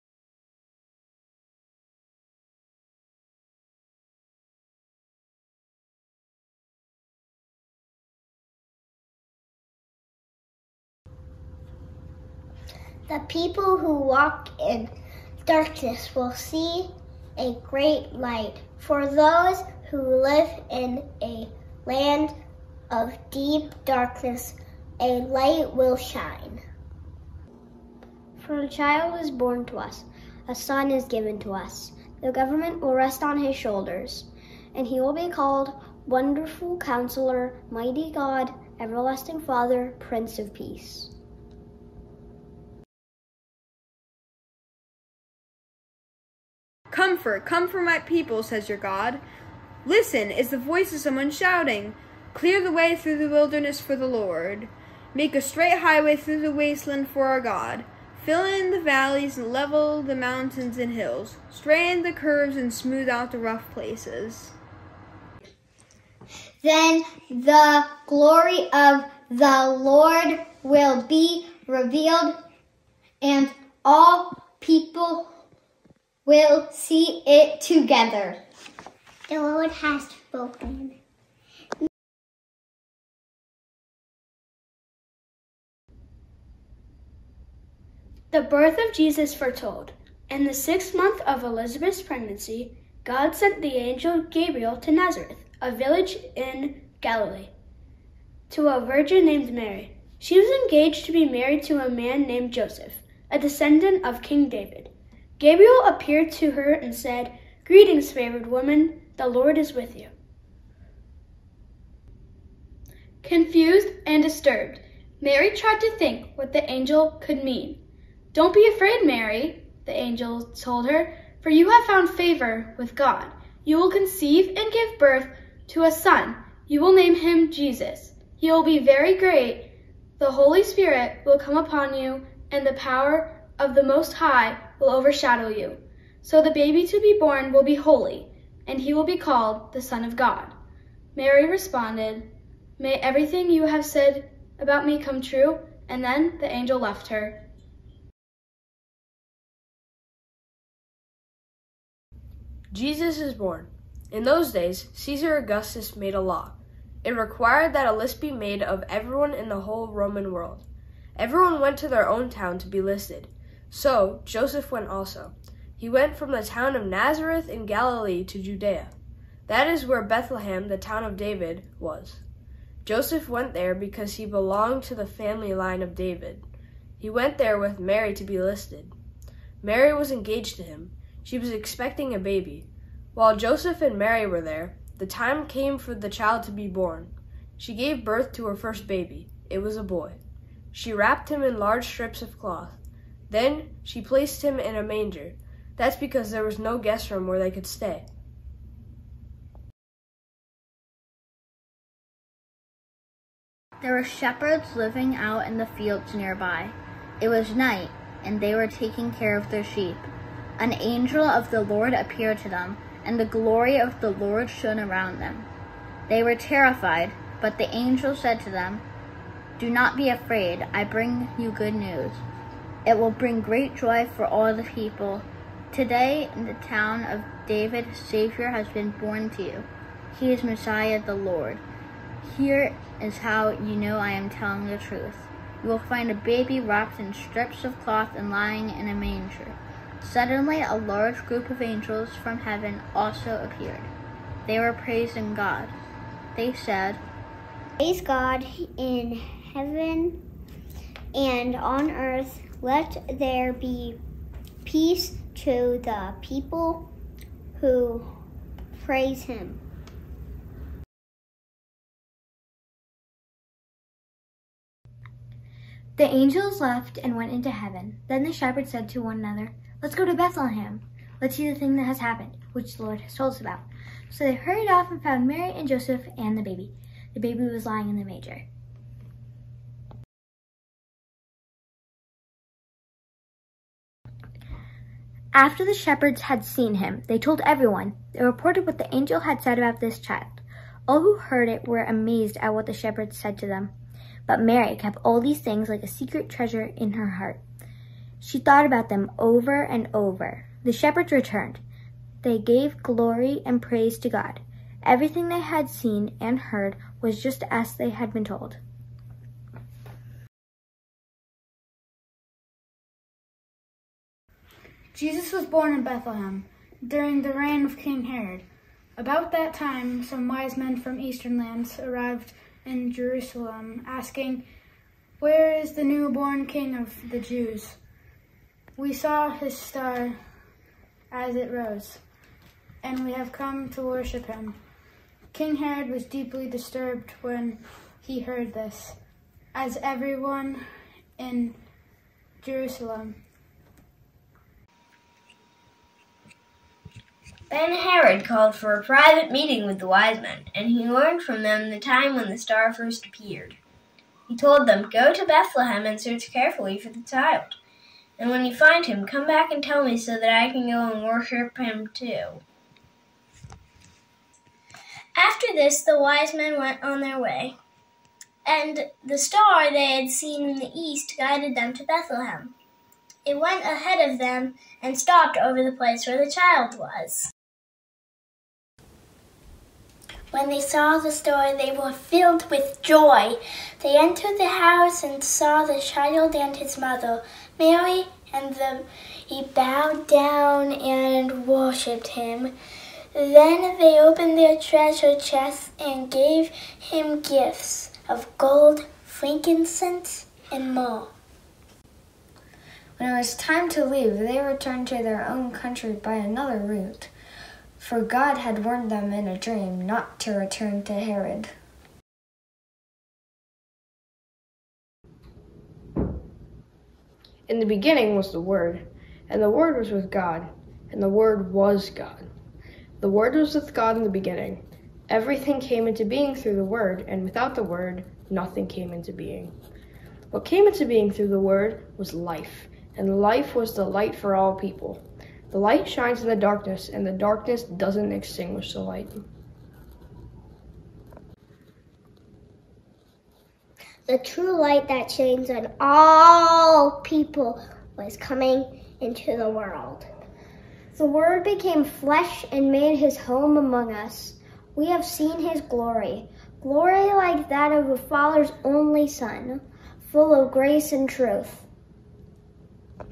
Lessons 2022
The children of Niantic Community Church share the Christmas story as part of our Lessons and Carols worship.